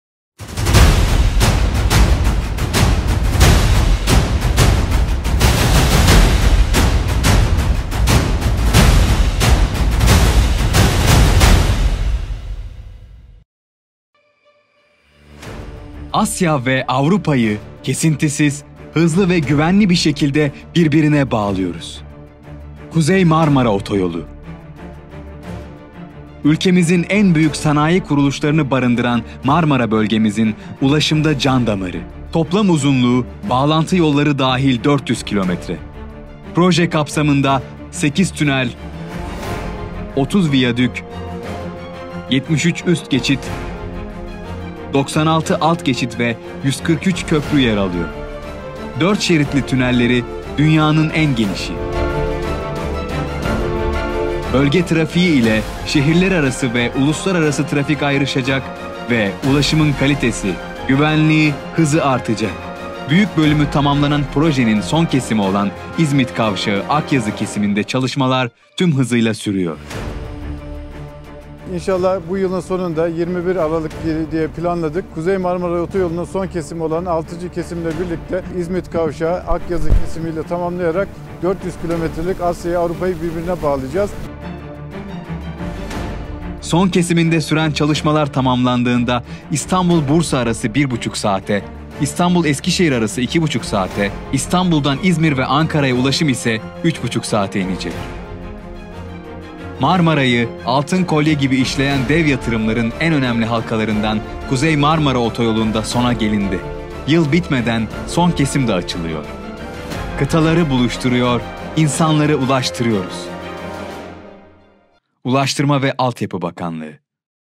Kuzey Marmara Otoyolu çalışmaları hızla ilerliyor.
Kıtaları buluşturmaya devam ediyoruz…
Dünyanın en geniş tünellerine sahip 400 km’lik Kuzey Marmara Otoyolu’nun son kesiminde çalışmalar hızla ilerliyor.

Üretimi ve ticareti canlandırarak, ekonomimizin büyümesine katkı sağlayacak Kuzey Marmara Otoyolu 2020 sonunda hizmete girecek.